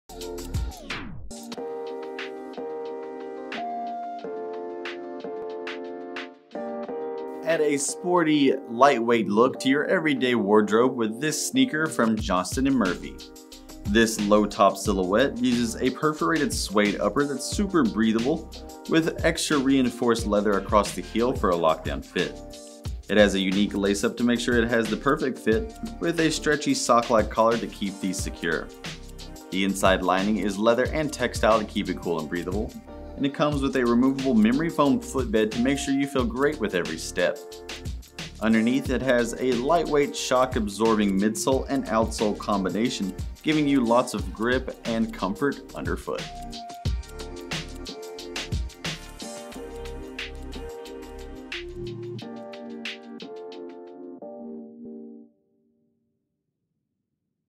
Johnston & Murphy Amherst Lace-Up SKU: 9578367
Add sport-inspired style to weekday looks with the expertly crafted Johnston &amp; Murphy&reg; Amherst Lace-Up sneaker.
Low-top silhouette with full lace clsoure.
Stitched pull-loop for easy on and off.
Perforated leather upper with elastic panel and reinforced leather heel.
Removable, memory foam cushioned insole.
Lightweight, shock-absorbing synthetic outsole.
Imported.